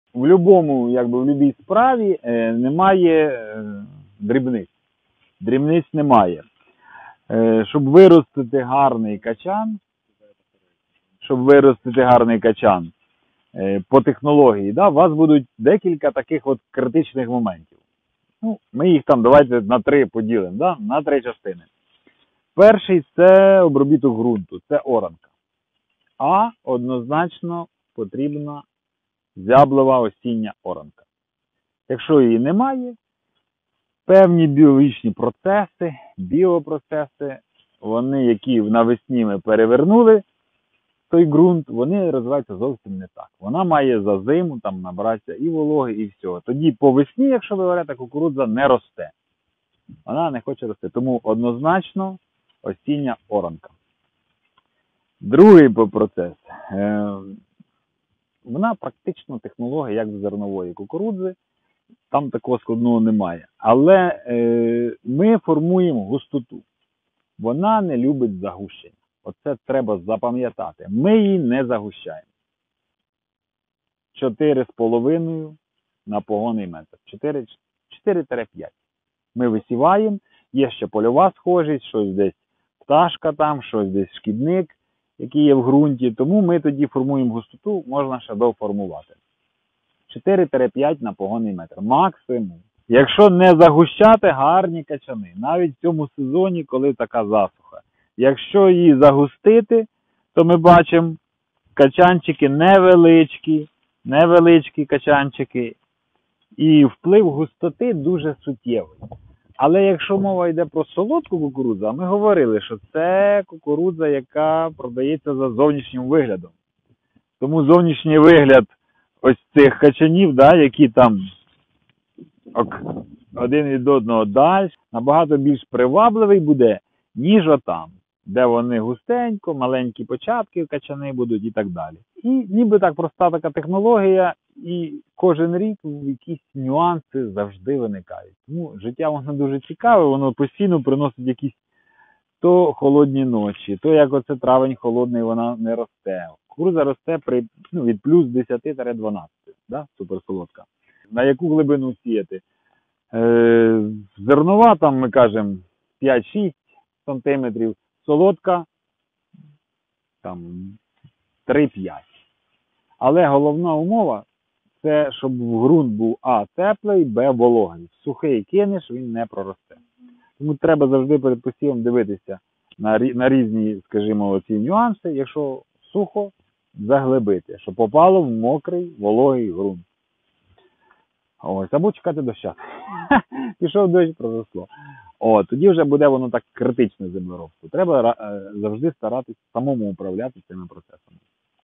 Як сіяти суперсолодку кукурудзу? 🌽
Як правильно посіяти суперсолодку кукурудзу? 🌽😍👍🏻
✅ Температура посіву +10-12°С.
✅ Не загущуйте ‼️ Оптимальна густота: 4-5 рослин у рядку на погонний метр. Міжряддя 70см.
✅ Яка вологість ґрунту?

🔴 Розповідає селекціонер Михайло НАГОРНЯК.